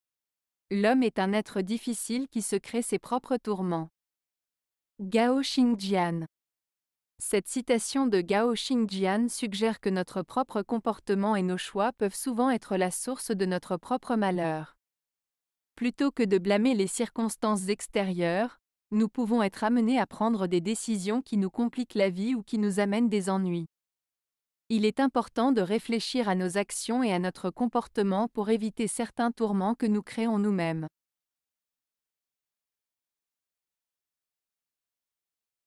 L'homme est un être difficile qui se crée ses propres tourments. - Gao Xingjian
Cette citation de Gao Xingjian suggère que notre propre comportement et nos choix peuvent souvent être la source de notre propre malheur. Plutôt que de blâmer les circonstances extérieures, nous pouvons être amenés à prendre des décisions qui nous compliquent la vie ou qui nous amènent des ennuis. Il est important de réfléchir à nos actions et à notre comportement pour éviter certains tourments que nous créons nous-mêmes.